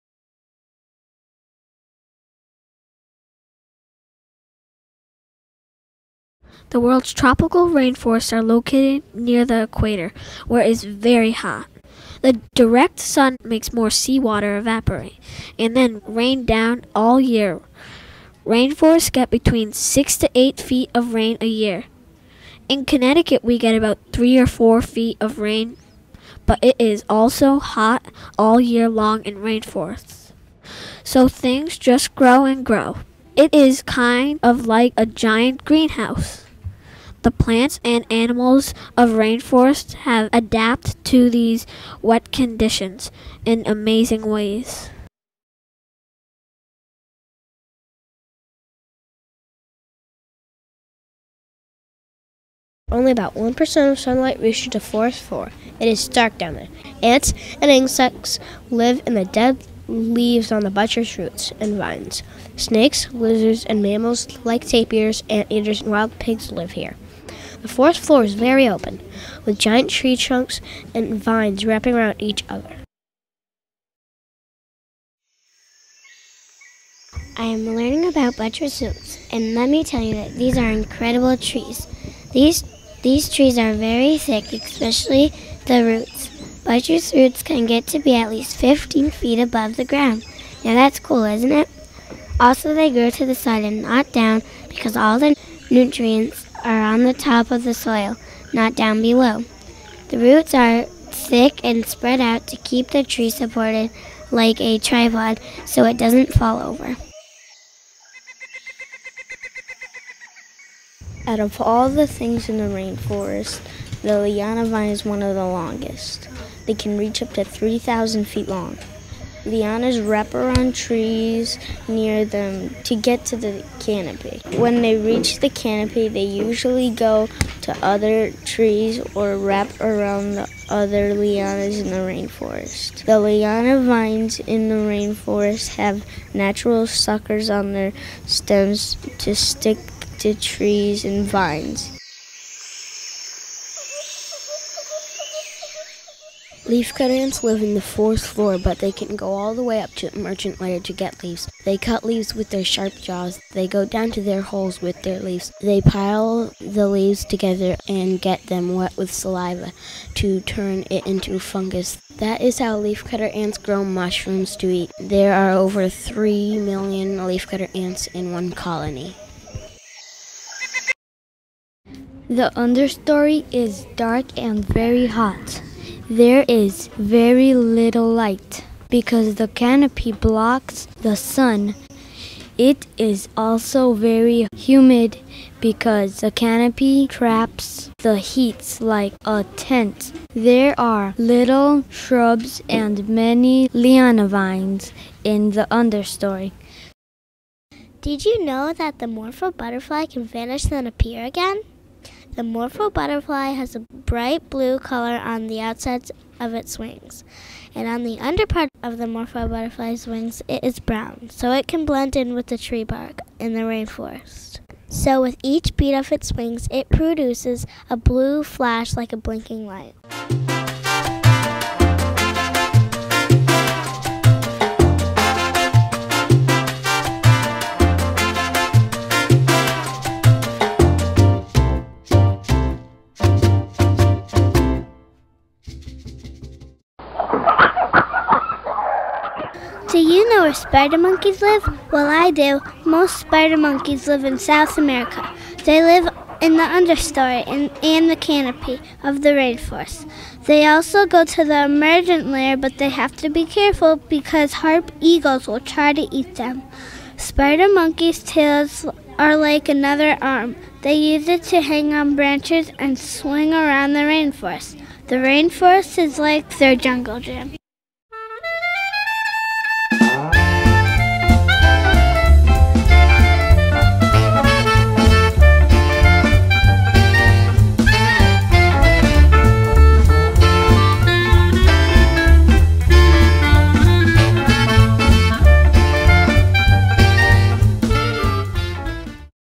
Rainforest Classroom part 1
Third grade students from Deans Mill School in Stonington, CT turned their classroom into a rainforest while studying habitats and adaptations. They offered guided, educational tours to other grades and raised over $600 dollars to protect rainforests through the Rainforest Alliance. Come for a virtual tour of their rainforest.